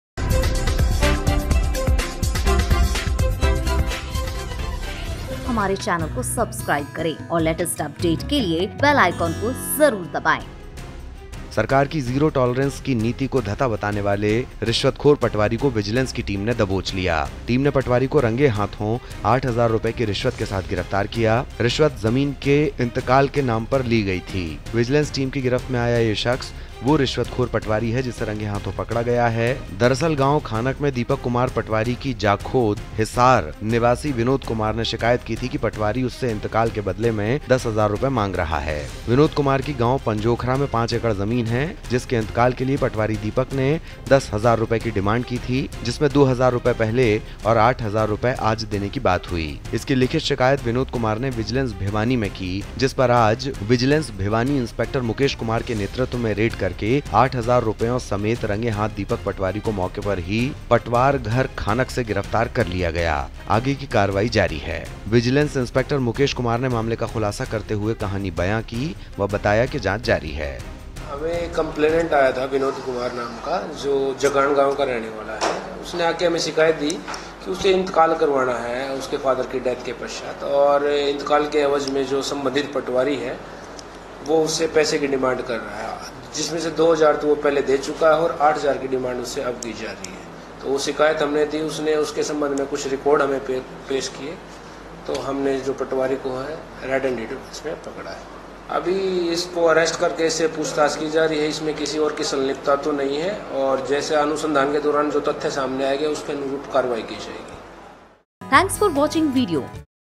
Bhiwani || रिश्वत लेते पटवारी गिरफ़्तार,किसान से की थी दस हज़ार की रिश्वत की माँग
सरकार की जीरो टोलरेंस की नीति को धत्ता बताने वाले कए रिश्वतखोर पटवारी को विजिलेंस की टीम ने दबोच लिया। टीम ने पटवारी को रंगे हाथों आठ हजार रूपये की रिश्वत के साथ गिरफ्तार किया। रिश्वत जमीन के इंतकाल के नाम पर ली गई थी।

Vo.1  विजिलेंस टीम की गिरफ्त में आया ये शख्स वो रिश्वतखोर पटवारी है जिसे रंगे हाथों पकड़ा गया है। दरअसल गांव खानक में दीपक कुमार पटवारी की जाखोद (हिसार) निवासी विनोद कुमार ने शिकायत की थी कि पटवारी उससे इंतकाल के बदले में दस हजार रूपये मांग रहा है। विनोद कुमार की गांव पंजोखरा में 5 एकड़ जमीन है जिस के इंतकाल के लिए पटवारी दीपक ने 10000 रूपये की डिमांड की जिसमें 2000 रूपये  पहले और 8000 रूपये  आज देने की बात हुई जिसकी लिखित शिकायत विनोद कुमार ने विजिलेंस भिवानी में की जिस पर आज विजिलेंस भिवानी इंस्पेक्टर मुकेश कुमार के नेतृत्व में रेड करके 8000 रूपयों समेत रंगे हाथ दीपक पटवारी को मौके पर ही पटवार घर खानक से गिरफ्तार कर लिया है। आगे की कार्यवाही की जा रही है।

Vo.2  विजिलेंस इंस्पैक्टर मुकेश कुमार ने मामले का खुलासा करते हुए कहानी बयंा की व बताया कि जांच की जा रही है।
बाईट:-मुकेश कुमार-विजिलेंस इंस्पैक्टर